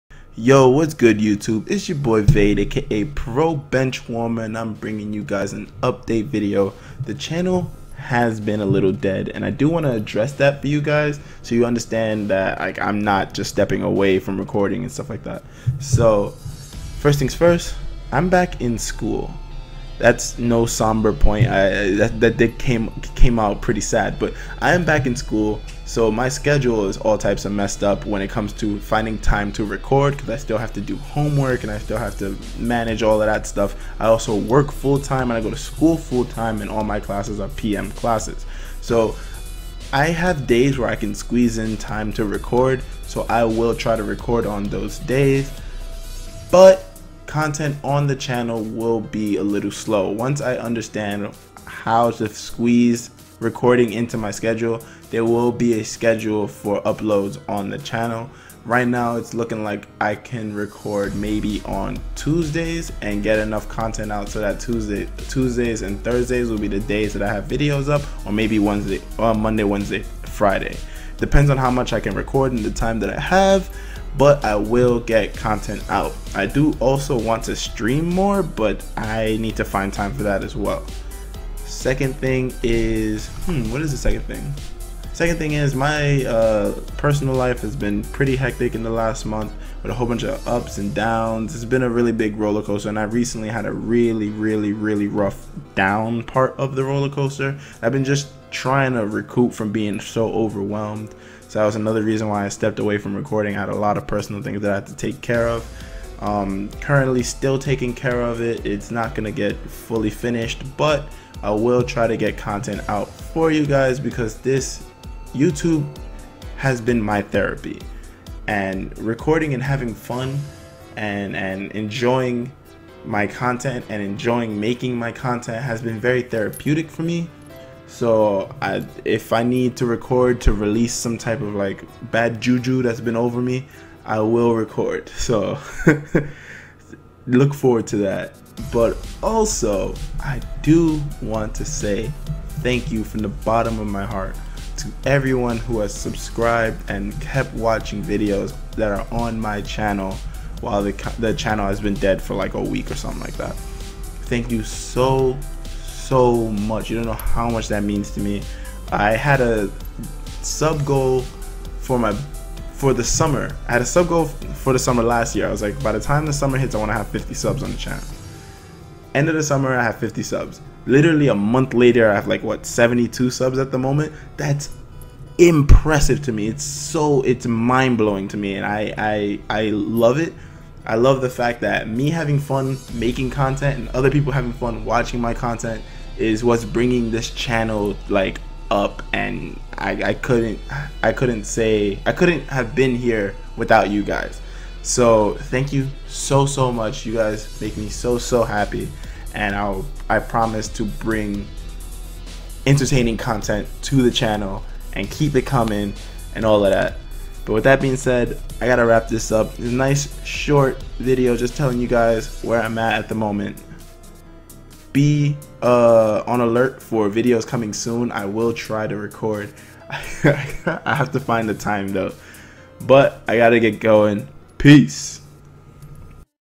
Where have you been, Vade?! | Update Video!
Sorry for abruptly stopping. Thank you for growing my channel. I love you all. I will still be here!